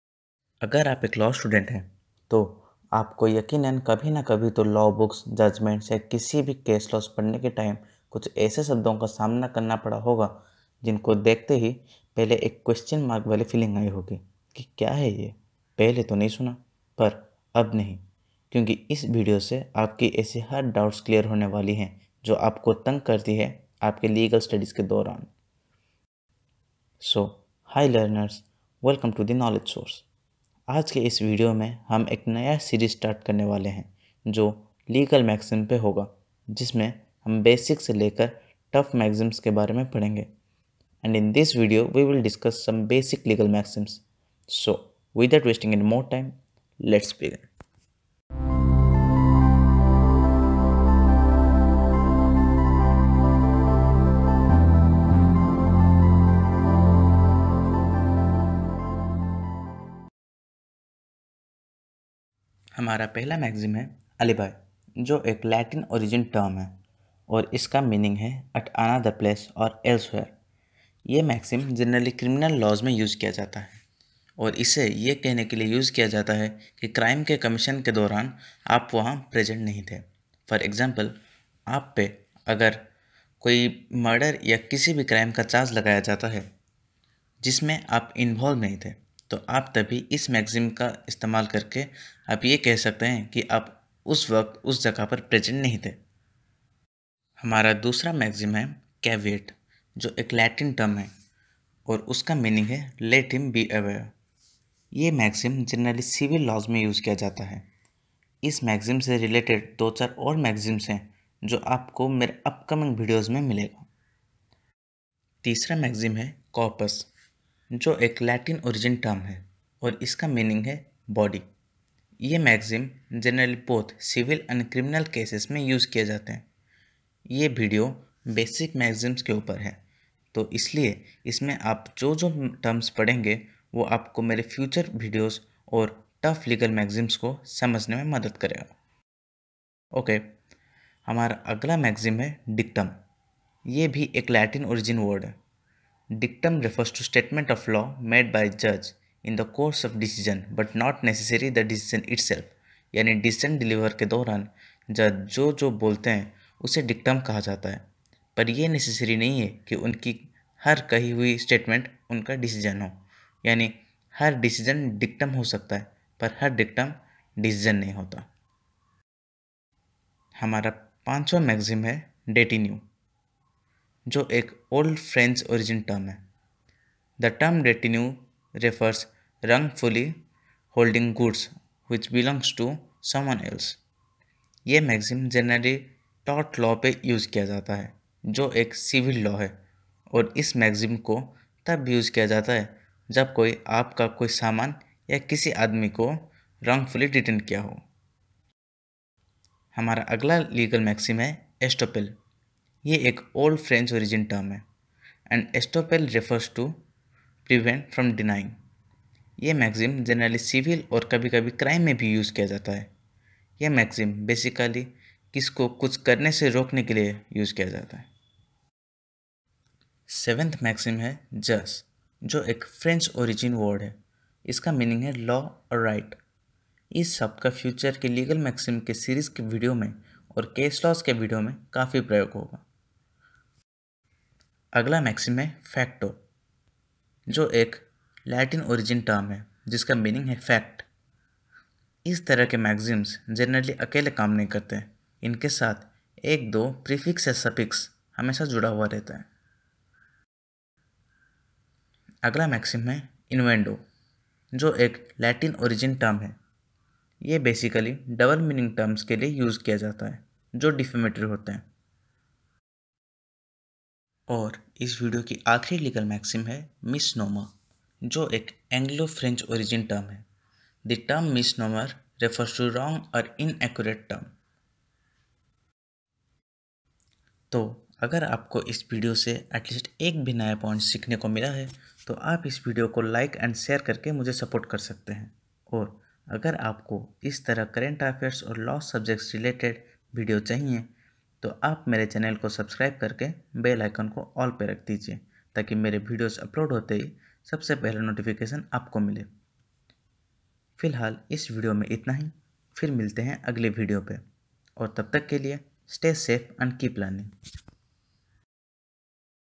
Boost Your Legal Understanding with Basic Legal Maxims #1 || In 5 Minutes
In this video, we are discussing 10 basic legal maxims which will boost your knowledge in legal maxim and legal studies.

Wooden Jhula
Metal Jhula
Jump rope
pull-up bar
Gym set